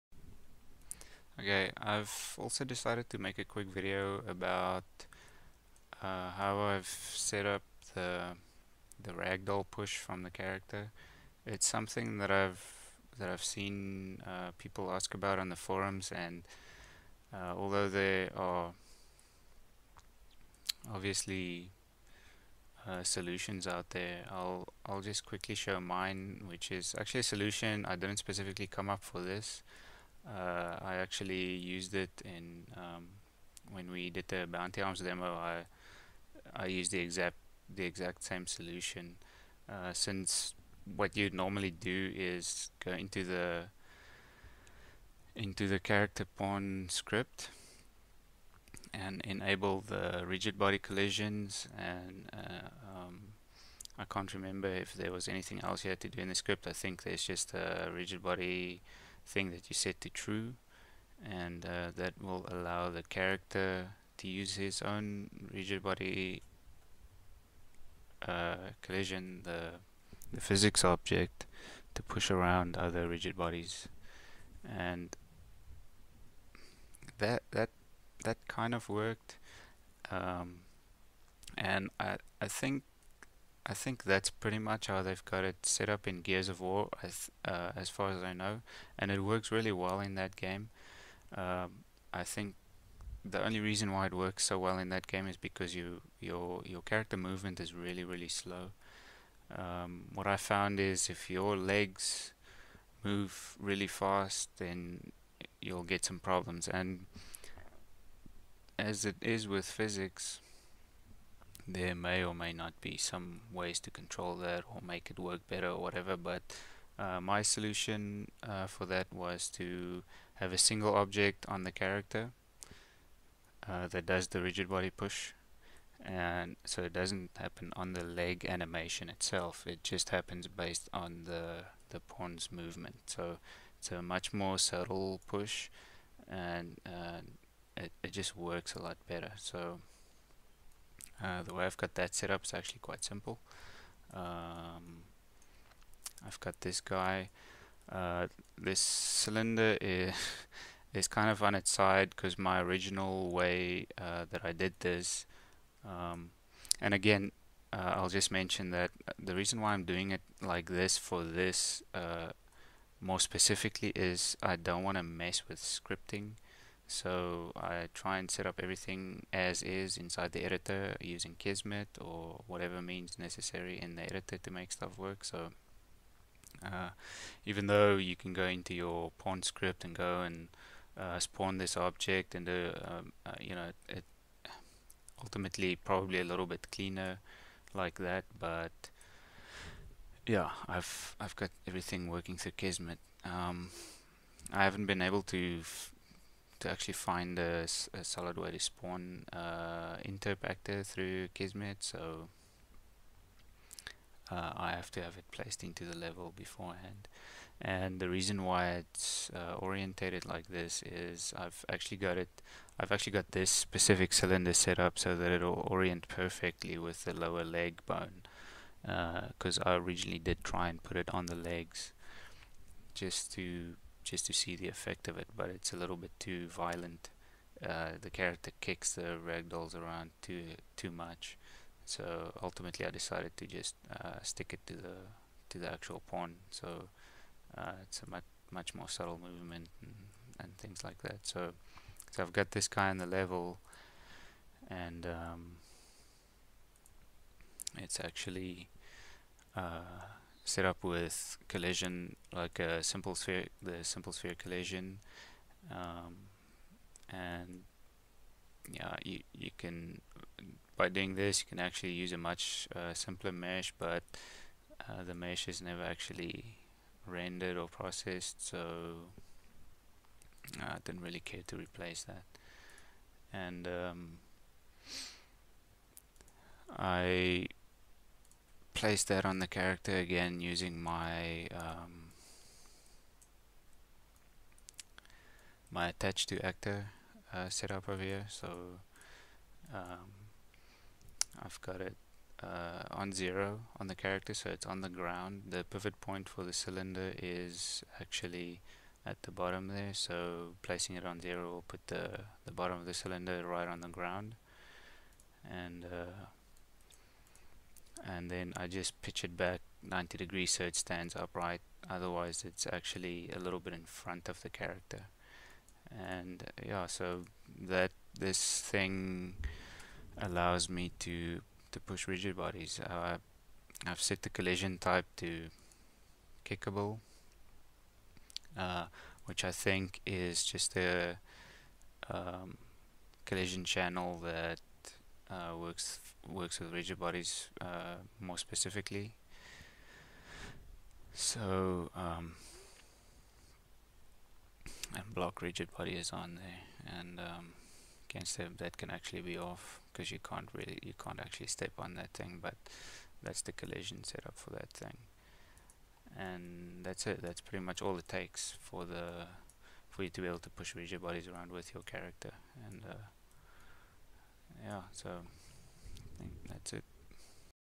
14 - UDK Game Using Kismet - by Obi-Wan
Part 14 of my game project work in progress using the UDK and Kismet.

- Setting up the ability for the player to push ragdolls around.